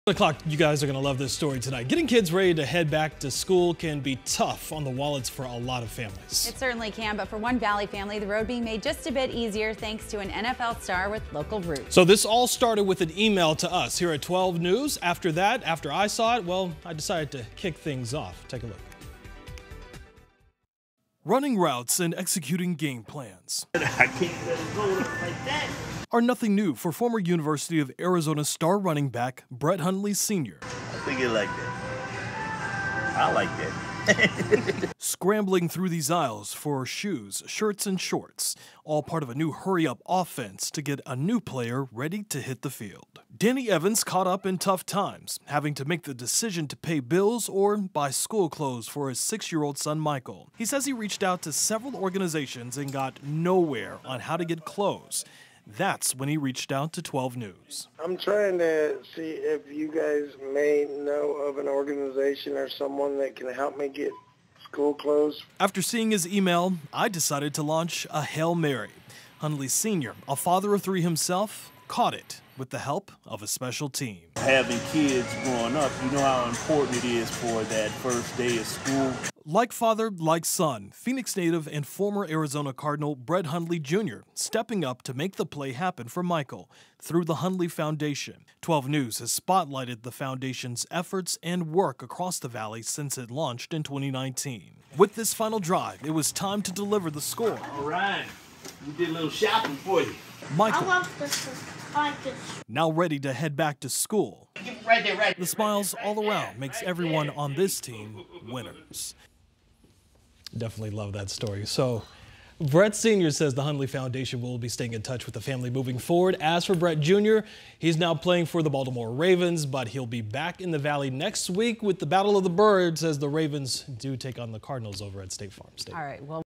Brett Hundley Sr. steps up to provide for a Valley family in need
Facing a blitz from the challenges and costs of preparing to send their son back to school, a Phoenix family threw a hail Mary pass for help and received a touchdown from an NFL star with ties to the Valley.

Danny Evans of Phoenix was recently looking for school clothes for his 6-year-old son, Michael.

Former University of Arizona star running back Brett Hundley, Sr. caught the pass with the help of a special team.